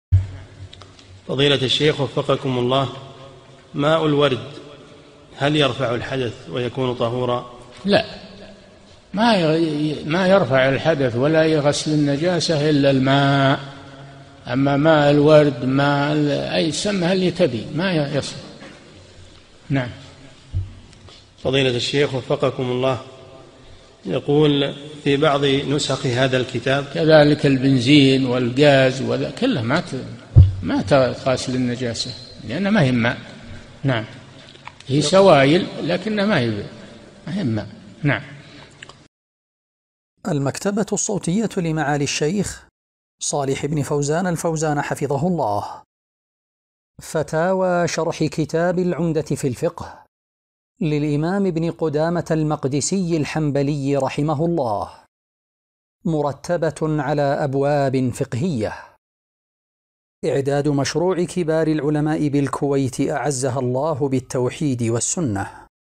[64 -3022] ماء الورد، هل يرفع الحدث ويكون طهورا؟ - الشيخ صالح الفوزان
-"[64 -3022] السؤال: ماء الورد، هل يرفع الحدث ويكون طهورا؟
الجواب: لا، ما يرفع الحدث ولا يغسل النجاسة إلا الماء، أما ماء الورد سمي الذي تبي ما يصلح، نعم، كذلك البنزين والجاز كله ما تغسل النجاسة، لأنها ما هي بماء، نعم، سوائل لكنها ما هي بماء، نعم.
للاشتراك في قناة الشيخ صالح الفوزان

للاستفسار والإقتراحات والدعم
رئيس الفريق
لمشروع كبار العلماء